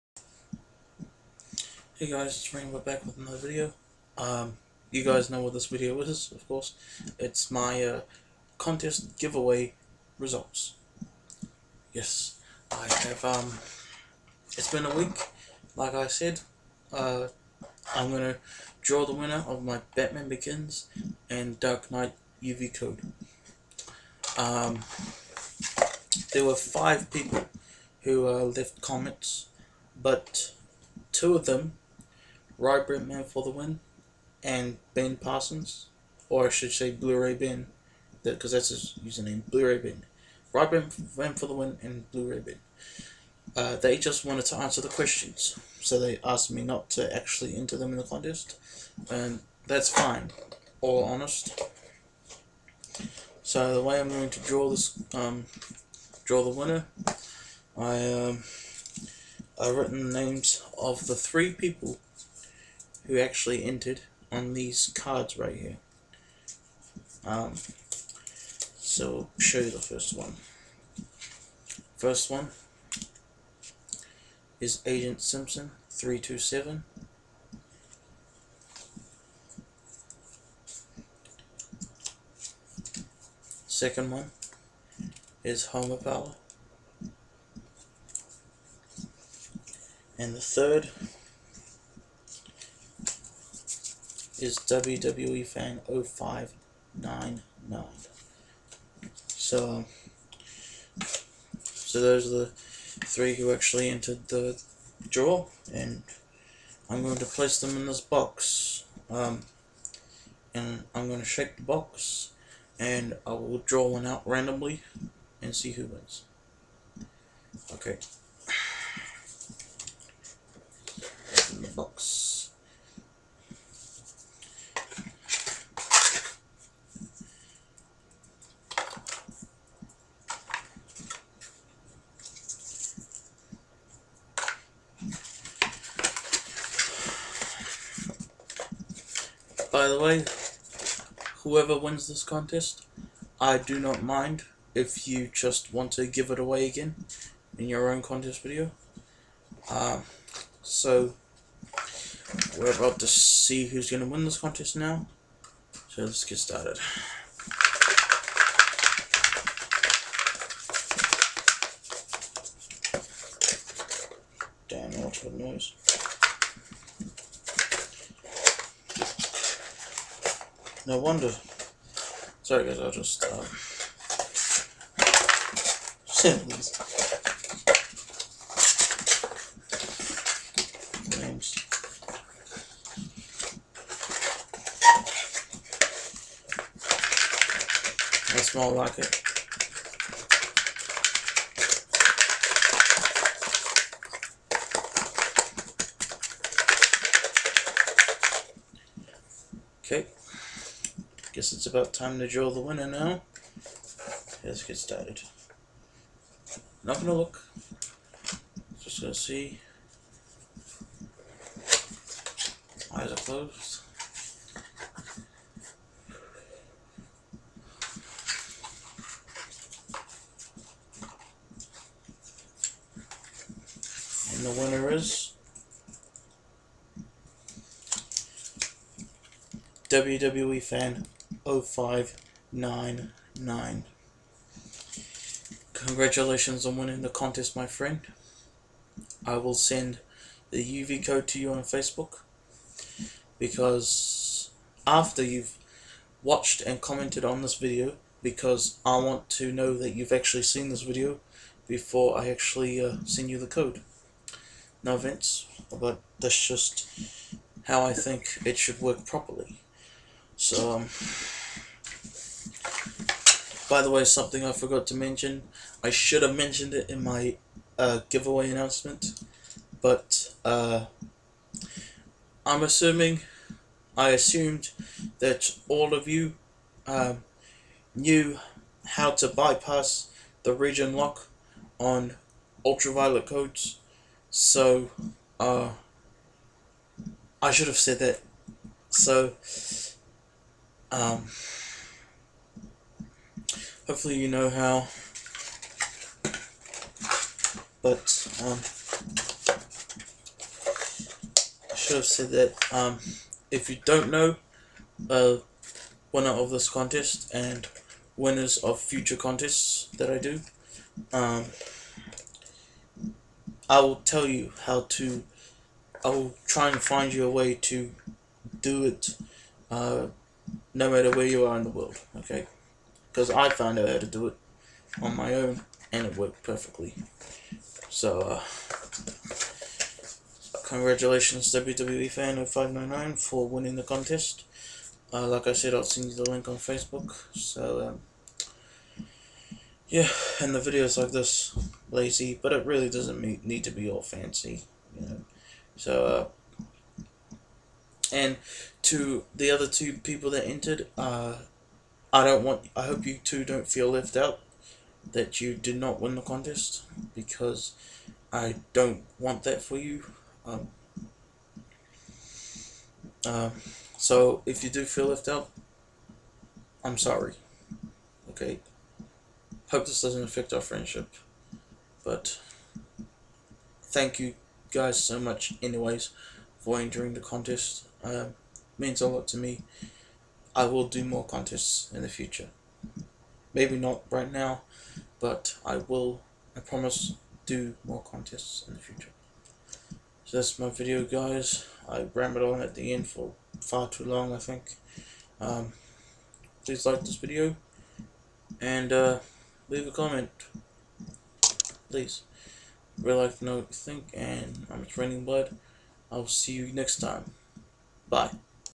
Batman Begins/The Dark Knight UV Giveaway WINNER
Thanks for entering this contest if you did, but who is the winner? Watch this video and find out.